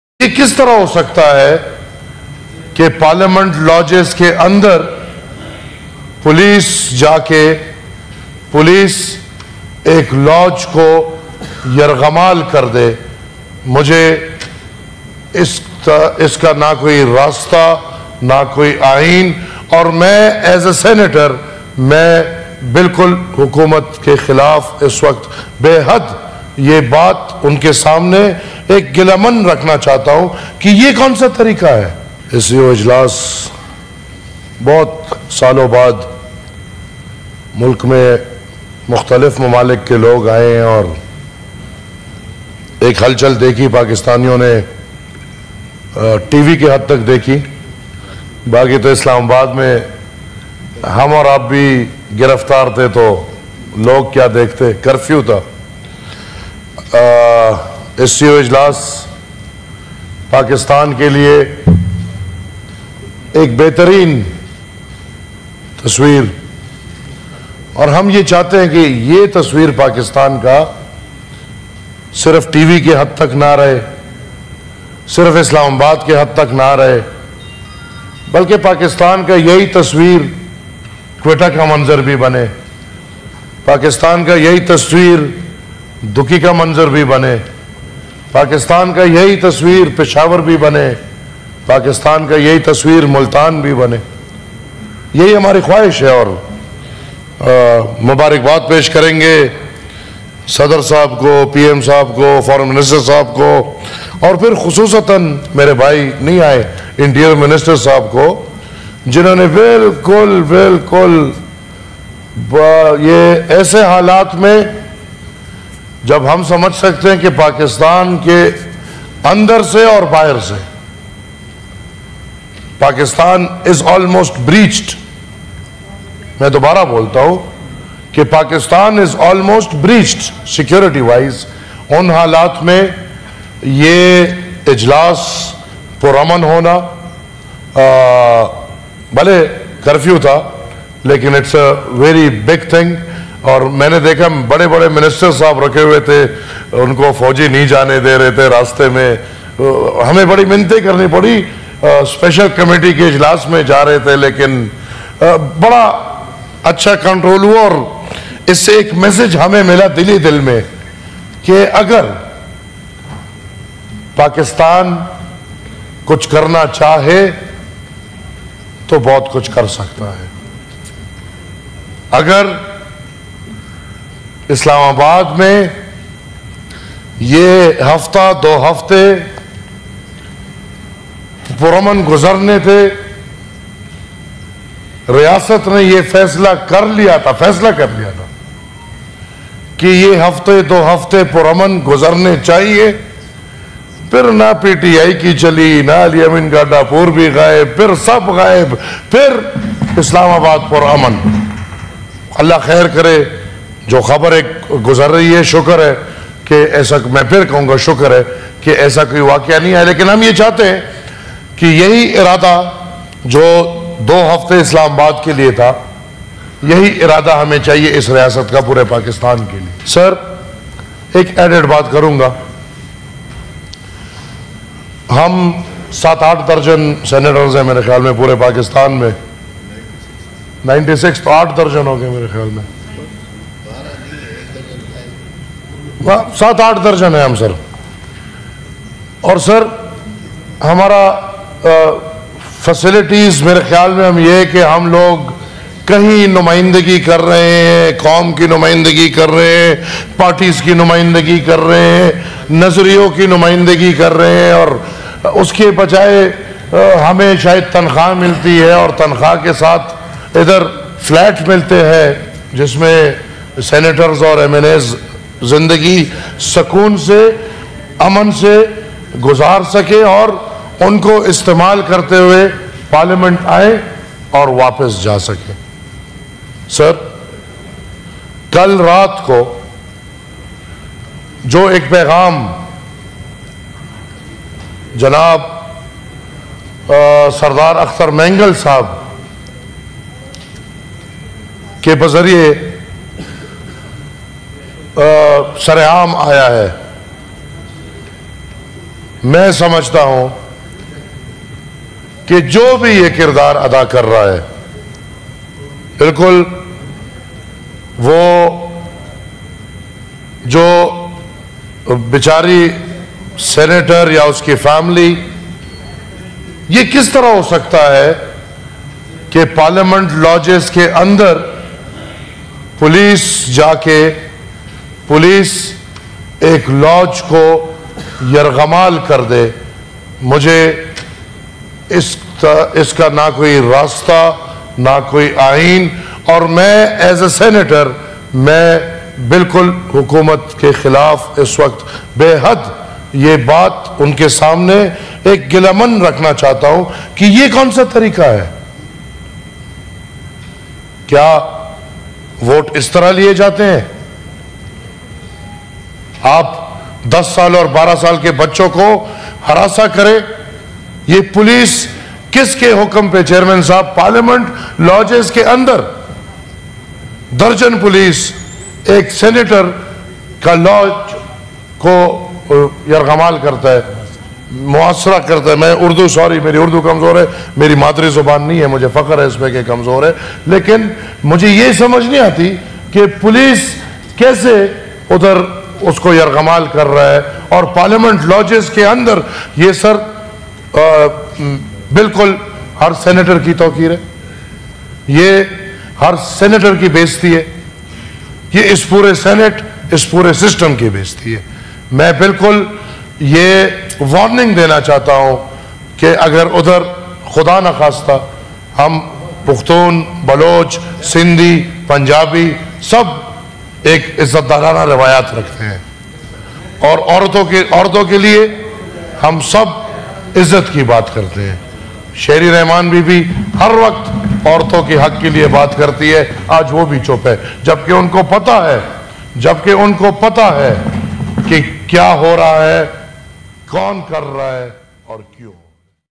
Aimal Wali Stands Firm | Advocating for Principles in Constitutional Amendment Debate | Khabarwalay
One of the largest online news platforms in Pakistan and worldwide, Khabarwalay provides exclusive news updates and the latest coverage.